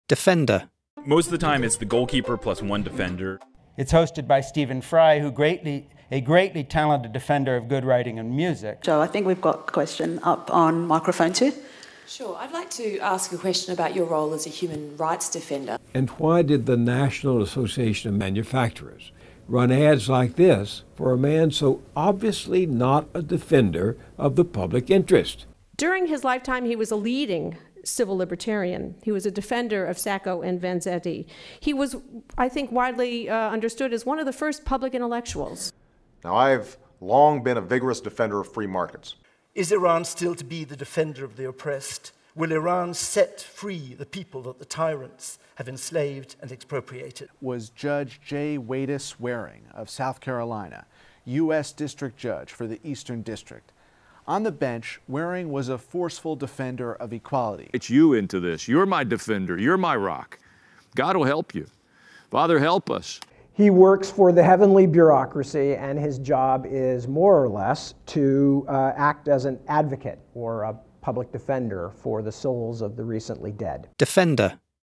The example of pronunciation of the word "defender"
Listen to various examples of the pronunciation of the word "defender"

Oxford dictionary. Level B2
londonis.ru